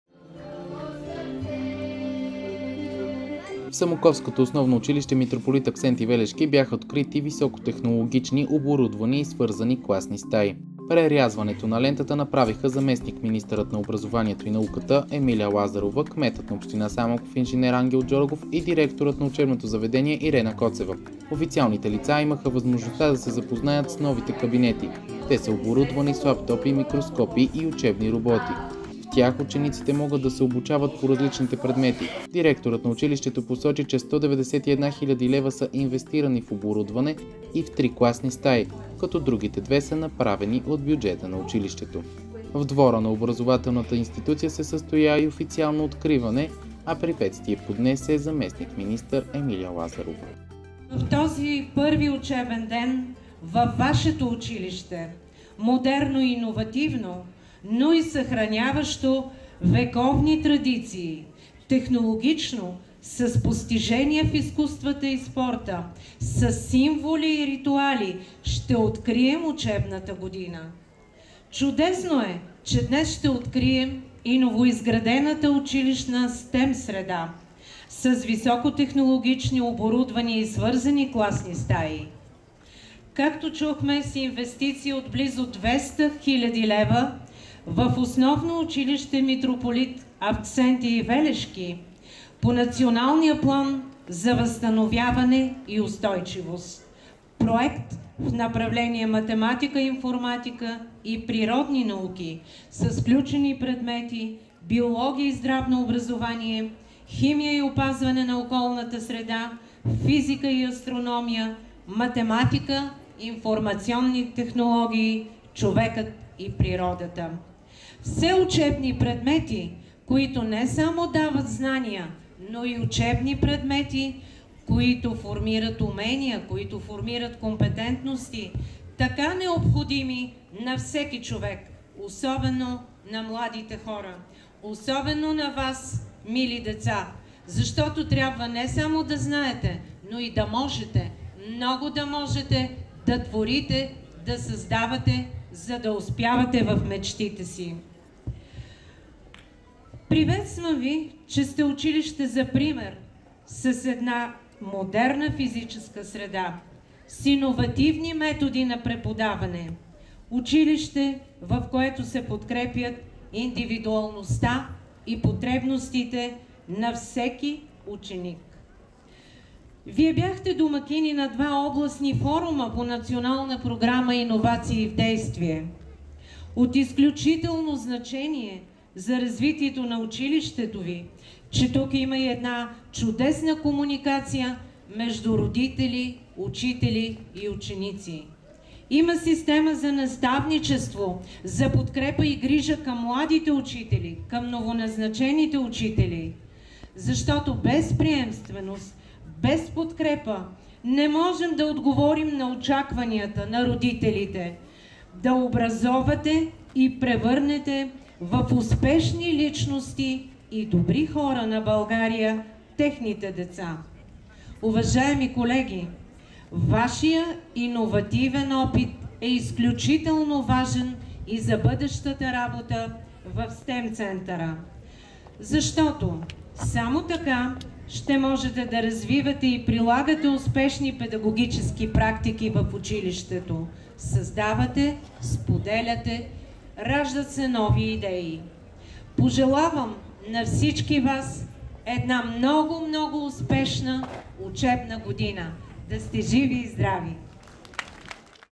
СТЕМ кабинети в самоковско училище откри заместник - министърът на образованието Емилия Лазарова
Заместник - министърът на образованието и науката Емилия Лазарова, кметът на община Самоков инж. Ангел Джоргов и директорът на Основно училище (ОУ) „Митрополит Авксентий Велешки“ прерязаха лентата на нови високотехнологични и оборудвани и свързани класни стаи. Те са пет брой и ще служат по различните учебни предмети – математика и информатика, биология и здравно образование, информационни технологии, природни науки.

Оборудвани са с учебни роботи, видео връзки, лаптопи и микроскопи, съобщи директорът Ирена Коцева. Тя посочи, че 191 хил. лева са инвестирани в оборудване и три класни стаи, като другите две са направени от бюджета на училището. СТЕМ кабинетите са изградени по Националния план за възстановяване и устойчивост.

Кабинетите бяха представени след официалното откриване на учебната година в двора на училището. Програмата за тържеството започна с изпълнение на песни, подготвени от първокласниците.

„Това е един от най-хубавите, най-милите празници във всяко българско семейство. За България това е празникът на просветата, празникът на образованието, затова днес празнуваме всички“. Това каза в официалното си приветствие заместник-министърът на образованието Емилия Лазарова пред ученици, учители и родители в двора на училището. Тя добави, че ОУ „Митрополит Авксентий Велешки“ пази вековни традиции и има високи постижения в изкуството и спорта.

Лазарова се обърна към учениците със съвета, че трябва не само да знаят, но и да могат да творят и създават, за да успяват в мечтите си. „Приветствам ви, че сте училище за пример с една физическа среда, с иновативни методи на преподаване, училище, в което се подкрепят индивидуалността и потребностите на всеки ученик“, посочи още заместник министърът.

„Без приемственост и подкрепа не можем да отговорим на очакванията на родителите да образовате и превърнете в успешни личности техните личности“, добави още тя.

Приветствие поднесе и кметът на община Самоков инж. Джоргов. Той пожела успехи на учениците и каза, че всички добри неща се случват заради добрата комуникация между училищното ръководство и Община Самоков.

Директорът Ирена Коцева пожела на всички ученици успехи и успешна учебна година.

Водосвет бе отслужен от архиерейския наместник на Самоковската духовна околия ставрофорен икономи Михаил Колев. Той прочете окръжното послание от името на Негово Светейшество Българският патриарх и Софийски митрополит Даниил.

Сред официалните лица бяха и началникът на Регионалното управление на образованието – София област Росица Иванова и проф. Найден Шиваров, директор на Национален СТЕМ център.

Репортер и оператор - Иван Ласкин
Монтаж - Ангел Николов